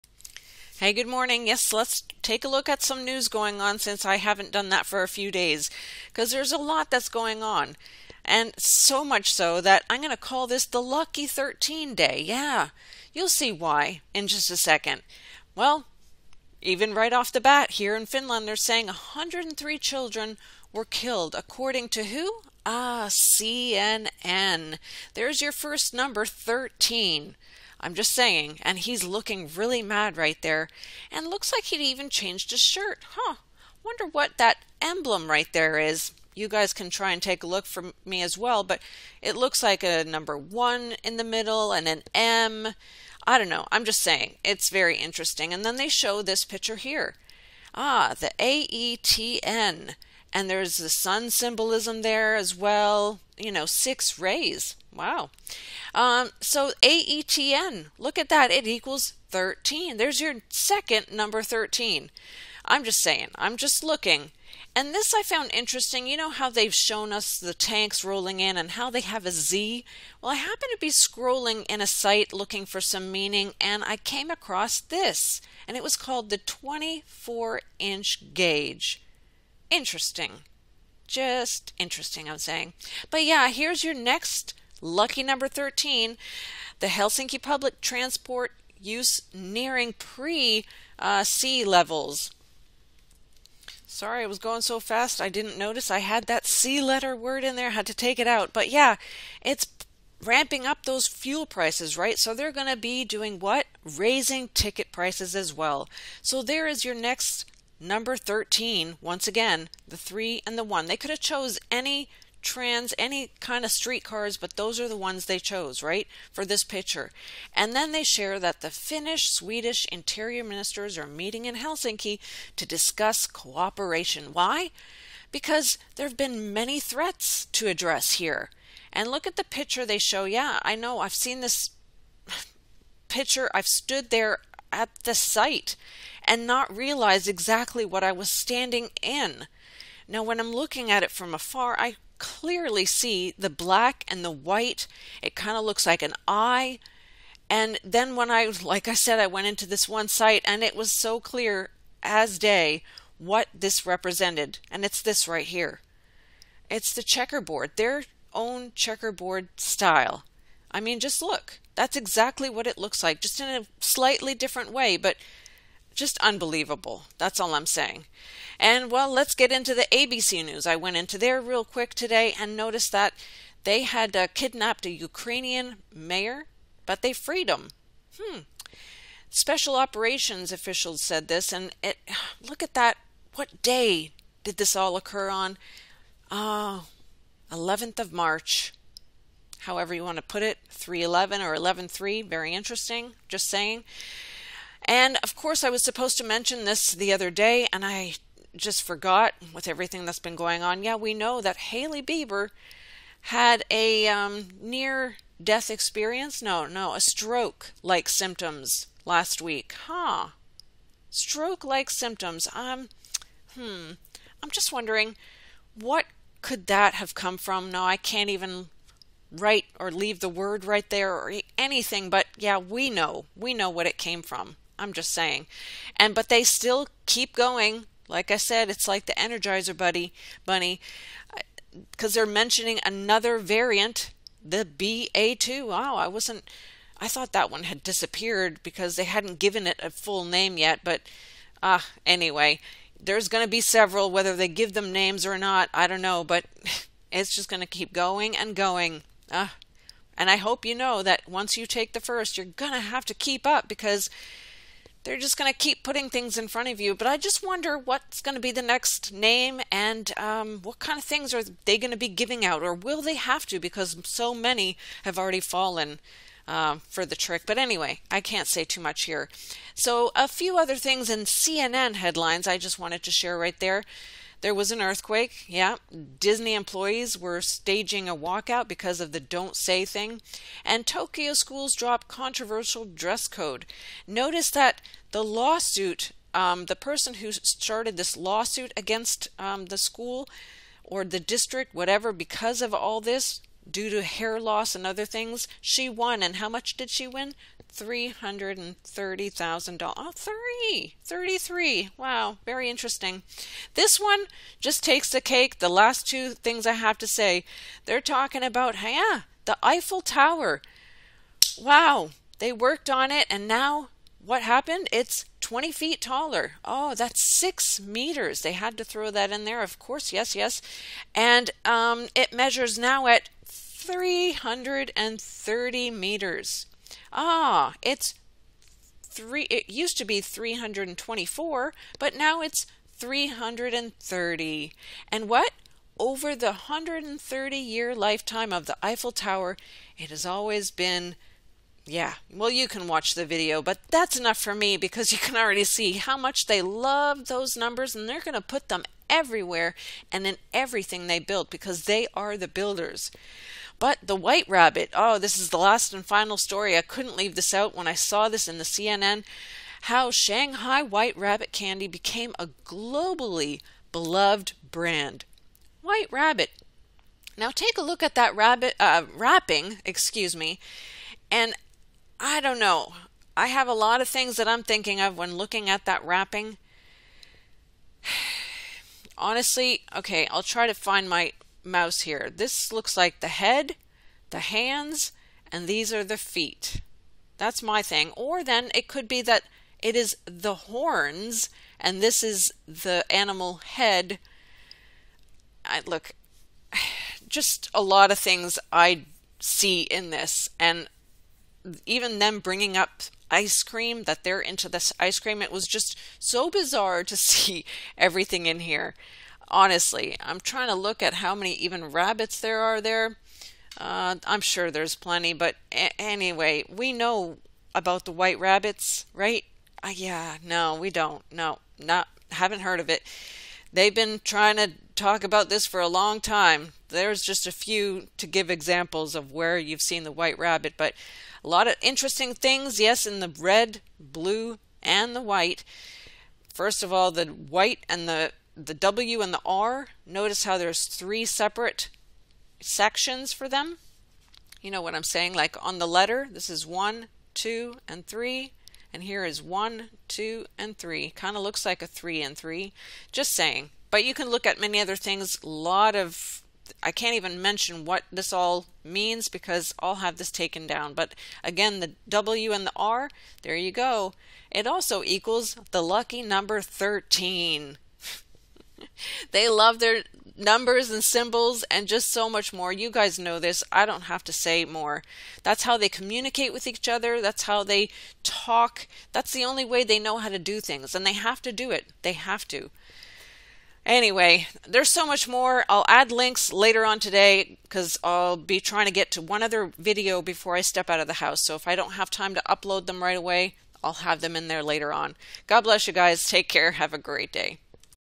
News (Lucky 13)
A brief look at the news around the world. (Topics; Ukraine, Finland fuel & threats, Bieber, Eiffel Tower & more) Test everything & trust Jesus alone. Do not live in fear. Have a great day!

Videos & Links used today
God bless y'all
Cindy 7grainsofsalt